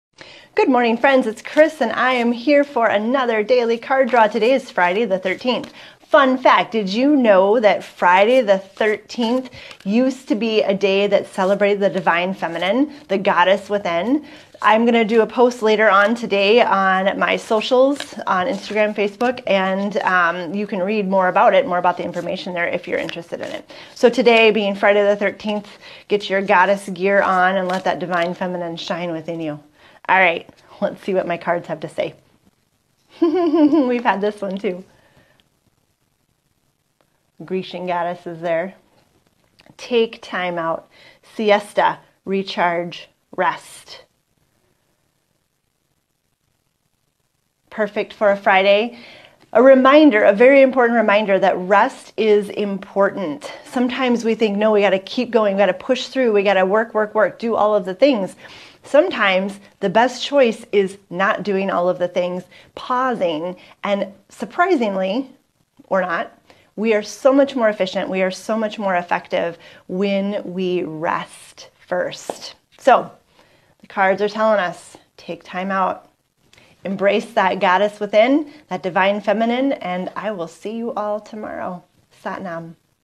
Daily Card Draw 10.13.23
Card: Get Into Nature

"Go outside and feel the sun on your face or the wind in your hair. Breathe and clean, fresh air, walk through riverbeds or feel the soft grass under your feet. Find balance and be one with a natural world."

xoxo